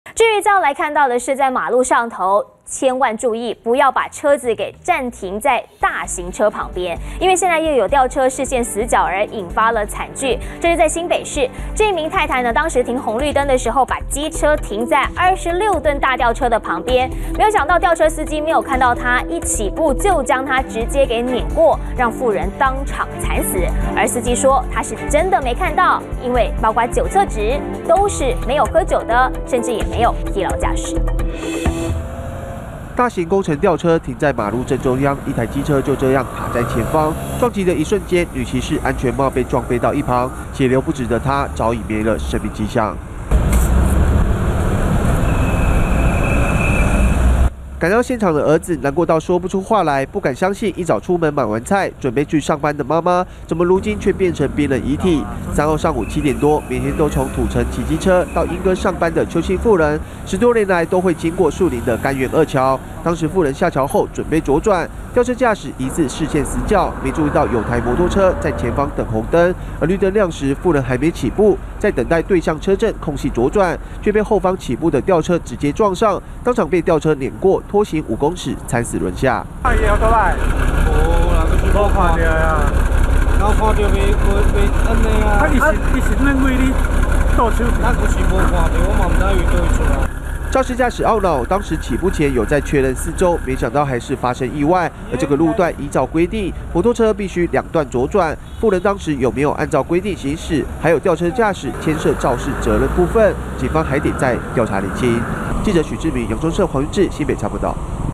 20141104中天新聞 疑視線死角釀禍 婦人遭吊車輾斃
新北市柑園二橋3號早上發生一起死亡車禍，當時60歲的邱姓婦人上班途中，騎車行經柑園橋準備左轉，後方吊車駕駛疑似視線死角，沒有注意到前方機車，當場追撞上去讓婦人慘死輪下。